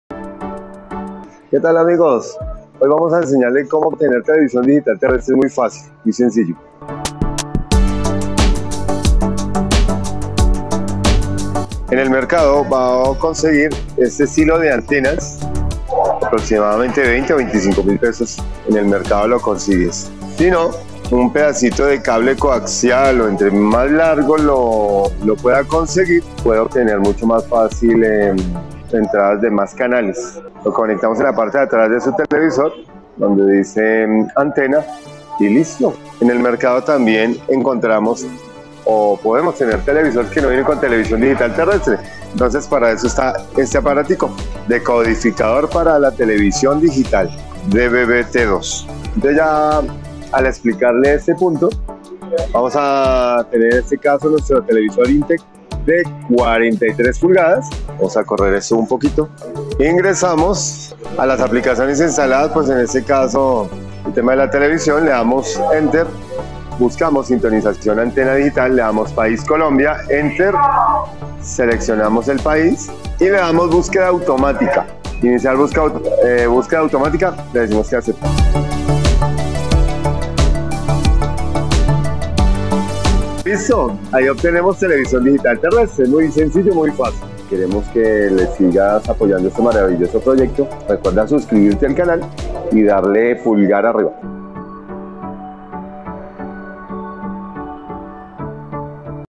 👉¿Cómo configurar la TDT?🤔
En este video te explicaremos como instalar la tdt, en este caso lo haremos en un  Smart TV INTEC que ya cuenta con ella (no importa la marca del TV, todos se configuran muy similar) o si no puedes usar un decodificador que lo puedes encontrar en nuestros almacenes.